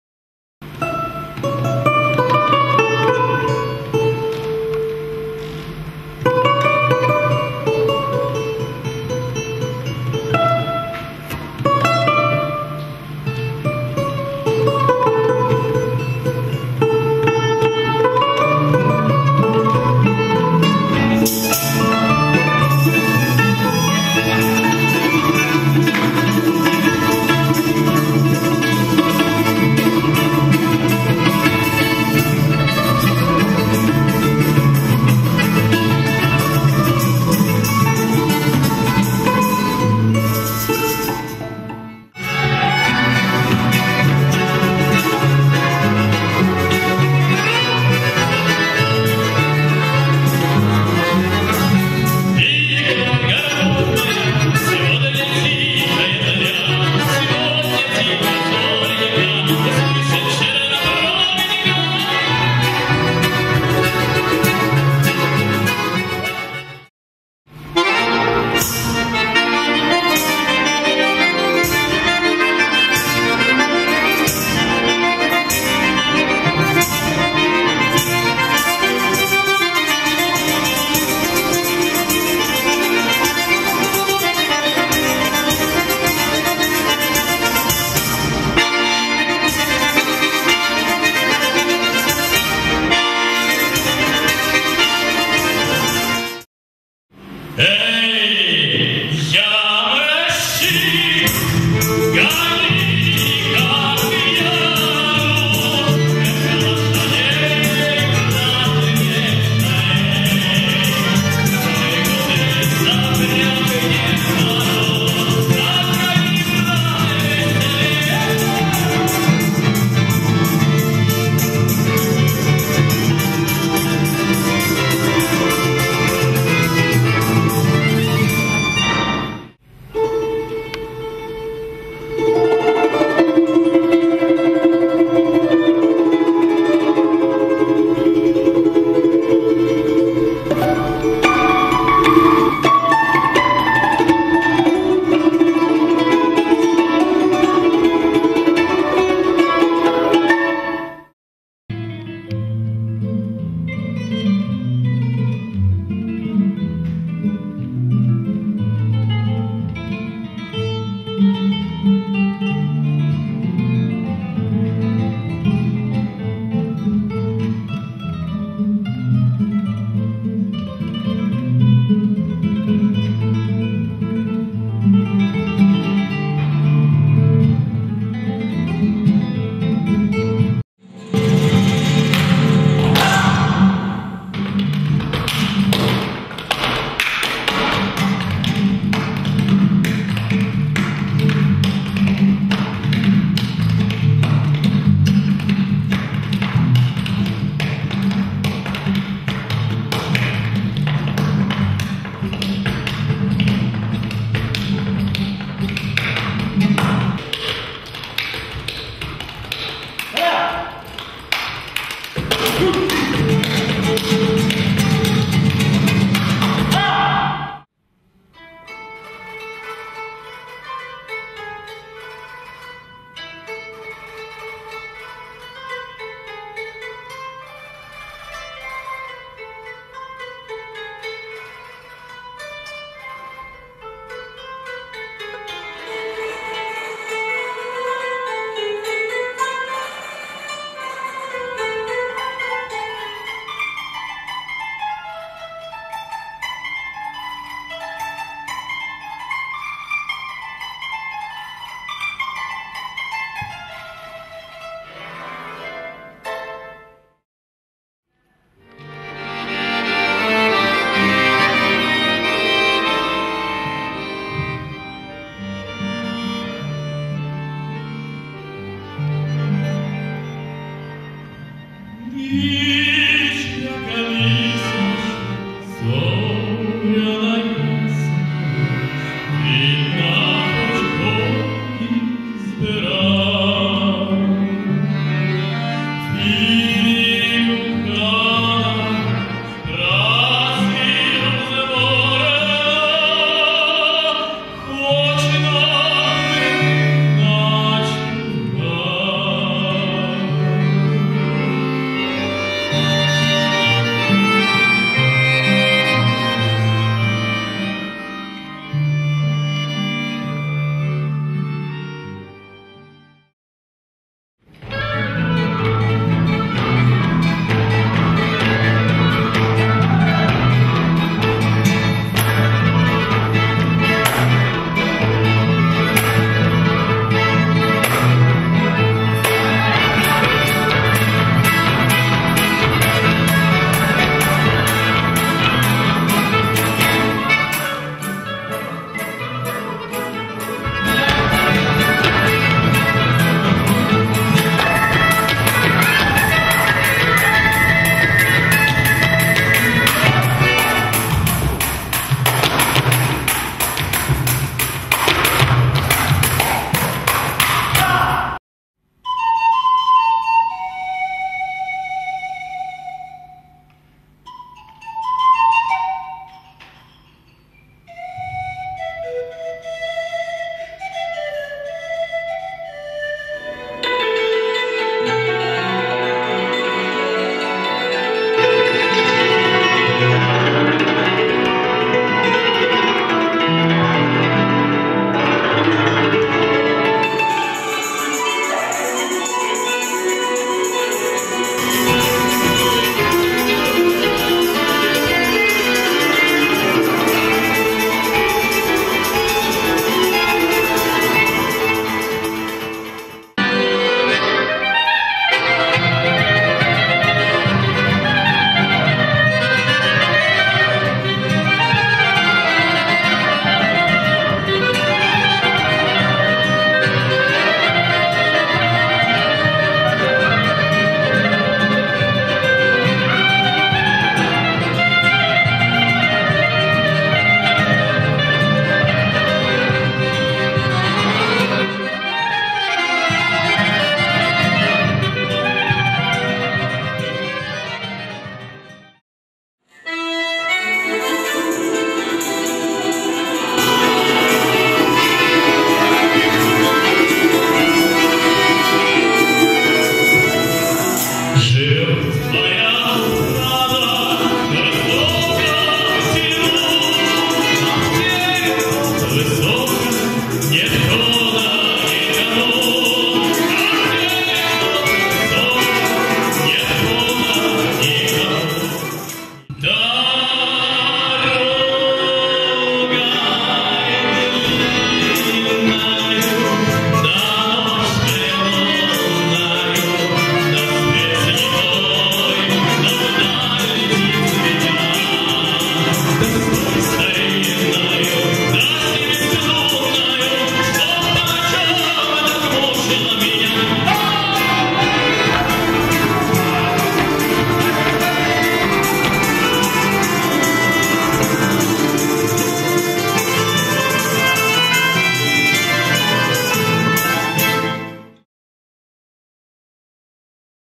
ВДНХ
Материалы любезно предоставлены в 2015 г. Юрой Кондратьевым после моей съемки его группы "Братина" с рекламной целью. Виртуоз русской балалайки заслуженный артист России Юрий Кондратьев и его коллеги. Еще много интересного увидите.